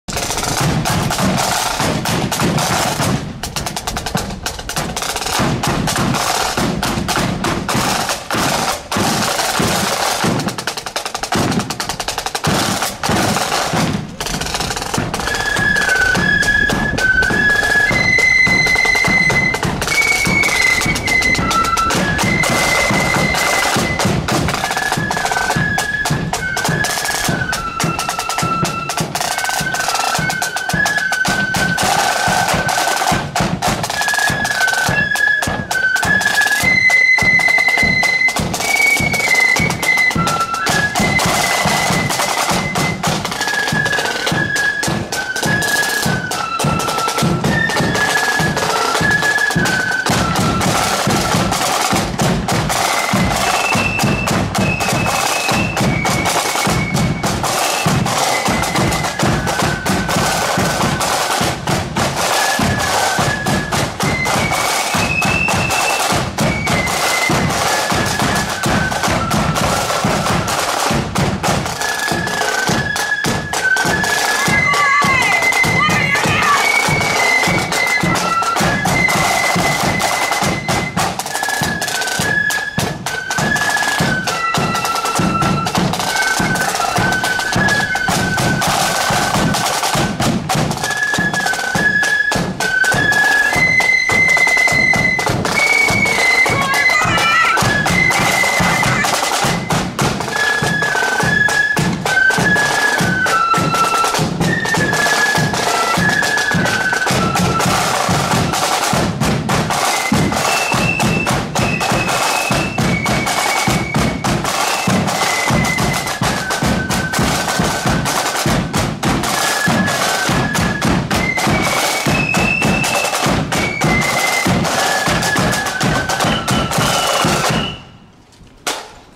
Randalstown S.O.U. (No.6) @ Remembrance Parade ~ Randalstown ~ 12/11/23 (4K)
Randalstown Sons Of Ulster Flute Band parading through Neillsbrook estate in Randalstown at the annual Remembrance Sunday parade held on Sunday 12/11/23.